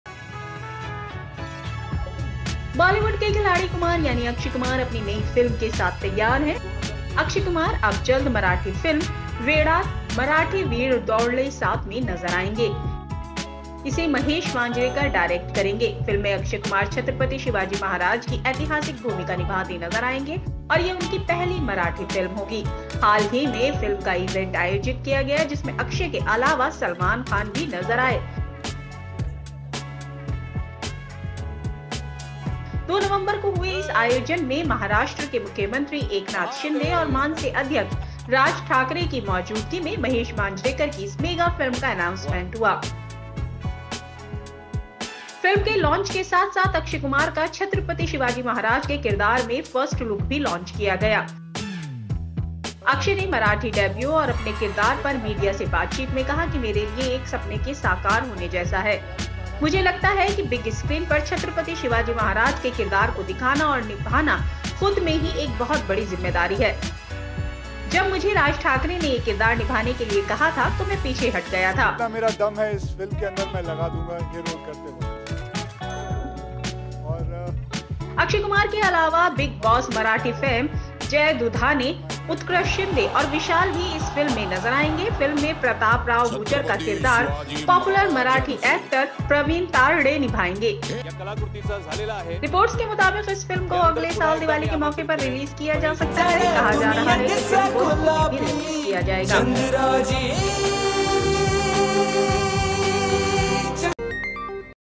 अक्षय कुमार का मराठी डेब्यू, निभाएंगे छत्रपति शिवाजी महाराज का रोल
डायरेक्टर महेश मांजरेकर की फिल्म का 'वेडात मराठे वीर दौडले सात' का बुधवार को महुरत रखा गया जिसमें सलमान खान समेत कई सेलेब्स ने शिरकत की.